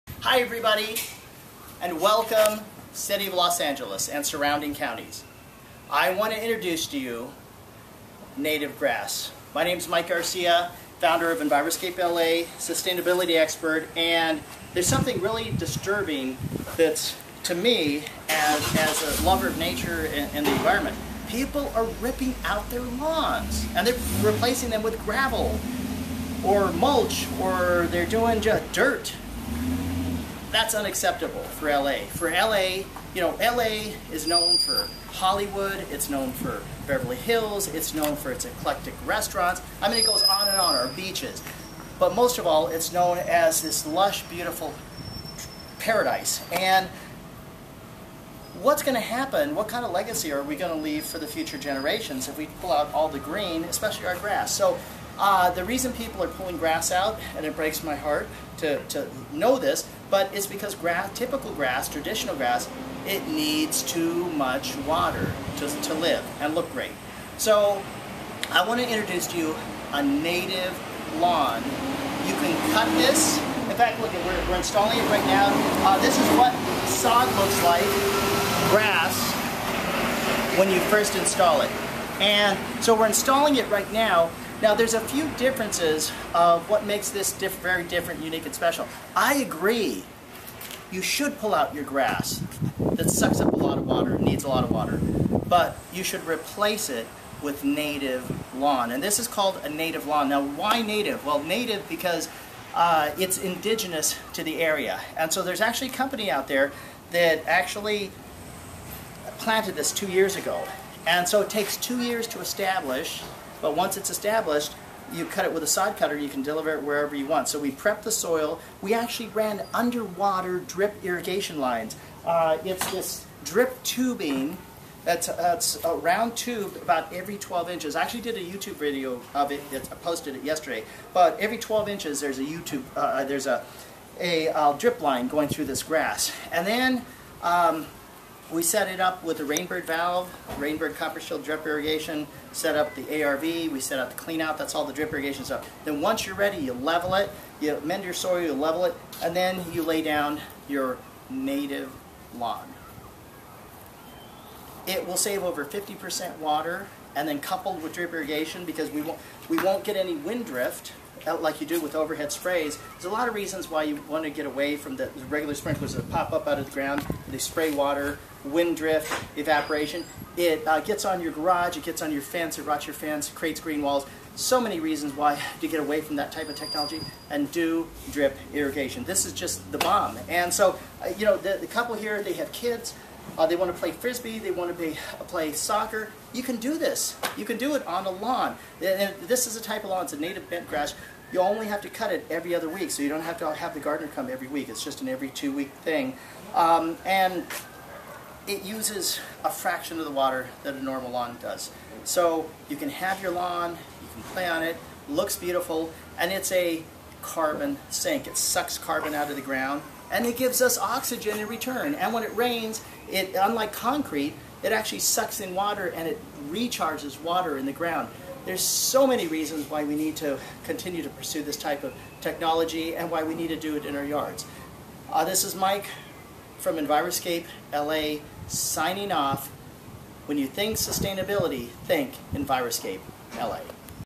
Introducing Drought Tolerant Lawn for California
Sustainability Expert Mike Garcia shows how you can fight the drought. with native grass which is drought tolerant, so will save lots of money on water and does not need to be cut as often. this lawn is watered with underground drip irrigation.